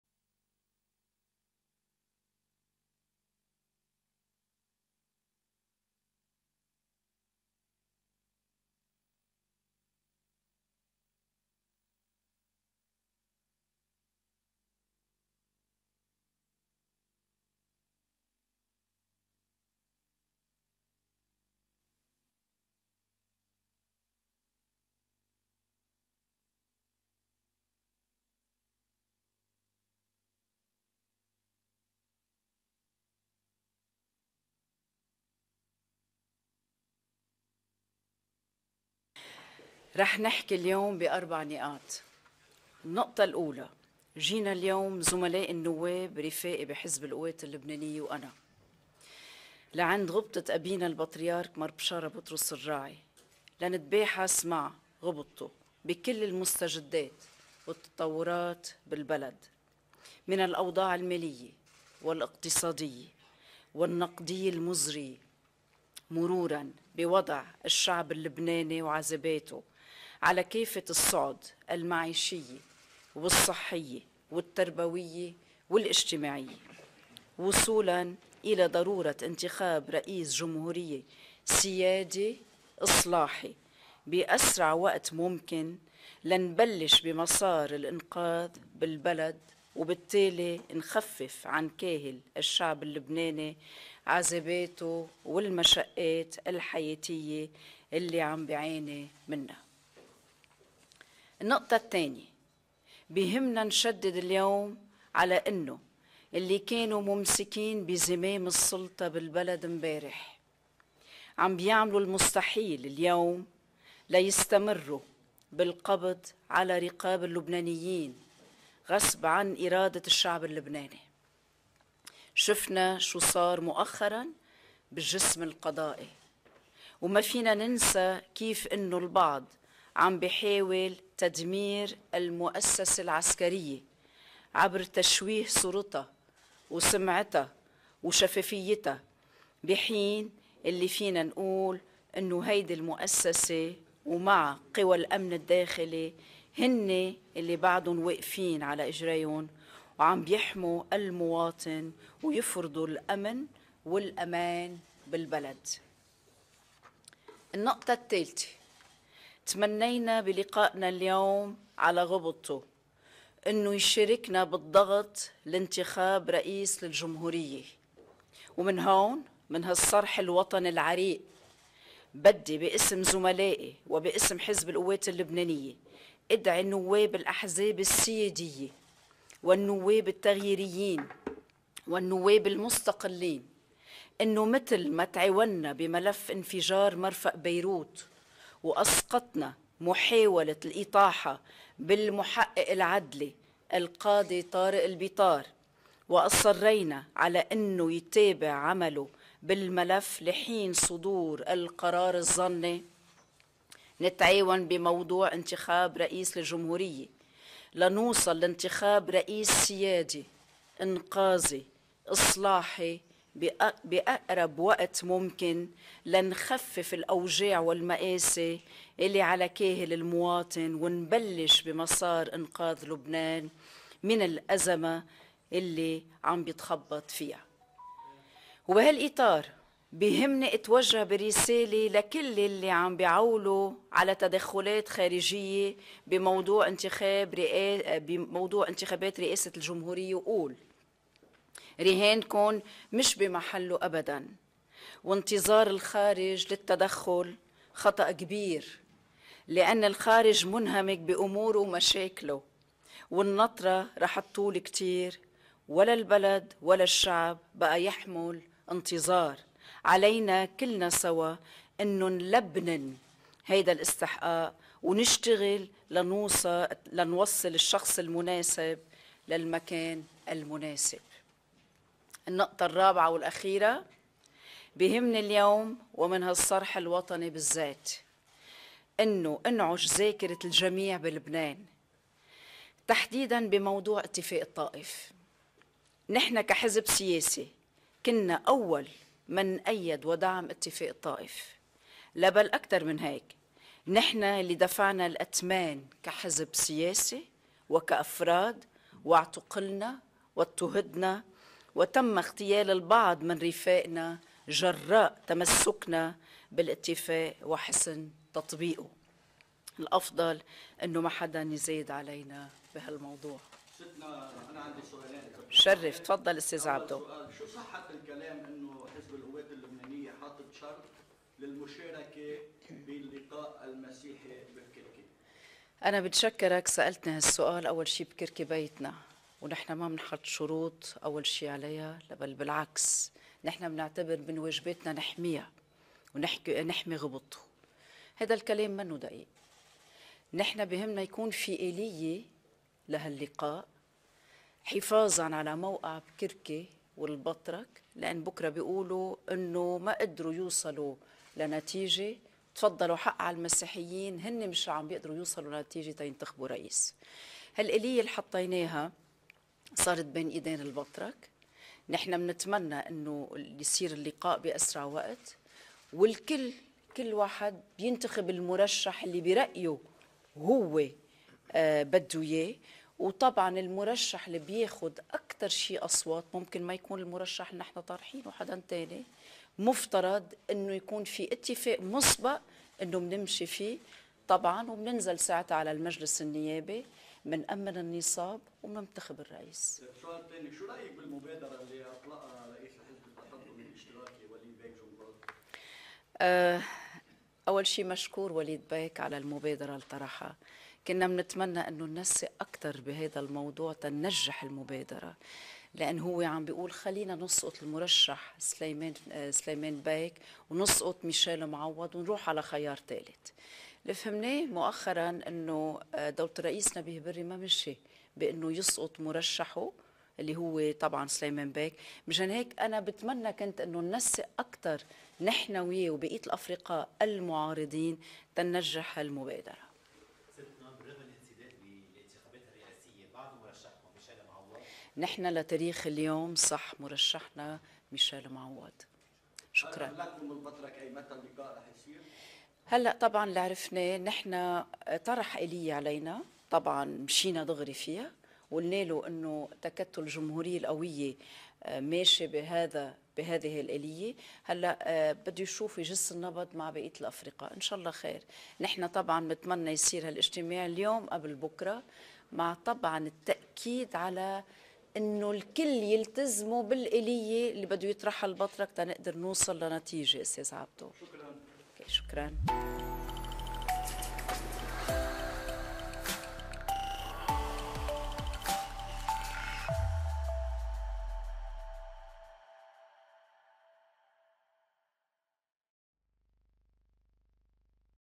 ستريدا جعجع : نحن لا نضع شروطاً على بكركي بل عرضنا تصورنا للخروج بنتائج إيجابية
زار وفد "تكتل الجمهورية القوية "، برئاسة النائبة ستريدا جعجع البطريرك الماروني الكاردينال مار بشاره بطرس الراعي في بكركي. ضم الوفد النواب : انطوان حبشي، الياس الخوري، رازي الحاج، جهاد بقرادوني، الياس اسطفان وسعيد الأسمر، بالإضافة إلى النائبين السابقين جوزيف اسحق وايدي أبي اللمع، ورئيس مكتب التواصل مع المرجعيات الروحية أنطوان مراد.

قالت النائبة جعجع، باسم الوفد: “جئنا اليوم، زملائي النواب ورفاقي في حزب القوات اللبنانية وأنا، إلى بكركي للبحث مع أبينا البطريرك الراعي، في المستجدات والتطورات في البلد، من الأوضاع المالية والاقتصادية والنقدية المزرية، مرورا بوضع الشعب اللبناني وعذاباته على الصعد كافّة، المعيشية والصحية والتربوية والاجتماعية، وصولاً إلى ضرورة انتخاب رئيس جمهورية سيادي إصلاحي بأسرع وقت ممكن، لنبدأ بمسار الإنقاذ في البلد، وبالتالي نخفِّف عن كاهل الشعب اللبناني عذاباته والمشقات الحياتية التي يعانيها".

أضافت: “يهمّنا أن نشدّد اليوم على أن من كانوا ممسكين بزمام السلطة في البلد في الفترة السابقة، يفعلون المستحيل اليوم ليستمروا بالقبض على رقاب اللبنانيين غصبا عن إرادتهم. ورأينا ما حصل أخيرا في الجسم القضائي. كما لا يمكننا أن ننسى كيف أن البعض يحاول تدمير المؤسسة العسكرية عبر تشويه صورتها وسمعتها وشفافيتها، في حين ما يمكننا قوله هو إن هذه المؤسسة، ومعها قوى الأمن الداخلي، هم الذين ما زالوا واقفين ويحمون المواطن ويفرضون الأمن والأمان في البلاد".

تابعت، “تمنّينا في لقائنا اليوم، على البطريرك الراعي، مشاركتنا بالضغط لانتخاب رئيس للجمهورية. من هنا، من هذا الصرح الوطني العريق، أودّ باسم زملائي وباسم حزب القوات اللبنانية، دعوة نواب الأحزاب السيادية والنواب التغييريِّين والنواب المستقلِّين، إلى أنه كما تعاونَّا في ملف انفجار مرفأ بيروت وأسقطنا محاولة إطاحة المحقق العدلي القاضي طارق البيطار وأصررنا على أن يتابع عمله في الملف لحين صدور القرار الظني، لنتعاون بموضوع انتخاب رئيس للجمهورية، لنصل لانتخاب رئيس سيادي إنقاذي إصلاحي في أقرب وقت ممكن، لنخفف الأوجاع والمآسي عن كاهل المواطن ونبدأ بمسار إنقاذ لبنان من الأزمة التي يتخبط فيها".

وقالت: "يهمني أن أتوجه برسالة إلى كل الذين يعوِّلون على تدخلات خارجية بموضوع انتخابات رئاسة الجمهورية، وأقول، رهانكم ليس في محلّه أبداً وانتظار الخارج للتدخل خطأ كبير، لأن الخارج منهمك بأموره ومشاكله والانتظار سيطول كثيرا، في حين لم يعد البلد ولا الشعب يحتملان الانتظار، وعلينا جميعاً أن نلبنن هذا الاستحقاق ونعمل لنوصل الشخص المناسب للمكان المناسب. يهمني اليوم، من هذا الصرح الوطني، أن أُنعش ذاكرة الجميع في لبنان، وتحديداً في موضوع اتفاق الطائف. نحن كحزب سياسي كنا أول من أيد ودعم اتفاق الطائف. لا بل أكثر من ذلك، نحن الذين دفعنا الأثمان كحزب وكأفراد، واعتُقلنا واضطُّهدنا وتمَّ اغتيال عدد من رفاقنا، جرّاء تمسكنا بهذا الاتفاق وحسن تطبيقه. والأفضل ألا يزايدن أحد علينا في هذا الموضوع”.

ورداً على أسئلة الصحافيين، قالت: “واجبنا حماية بكركي ونحن بالتأكيد لا نضع شروطاً بل عرضنا تصورنا للآلية المناسبة للخروج بنتائج إيجابية من الاجتماع المقرر انعقاده”.

وشكرت لرئيس الحزب التقدمي الاشتراكي وليد جنبلاط مبادرته، وقالت: "تمنينا لو نسق معنا لكانت نجحت وخصوصاً أننا علمنا أن رئيس مجلس النواب نبيه بري لم يوافق على التخلي عن اسم سليمان فرنجية”.

وشددت على أن “مرشحنا هو ميشال معوّض لتاريخ اليوم وعلى الجميع التزام الآلية التي سيضعها البطريرك للاجتماع”.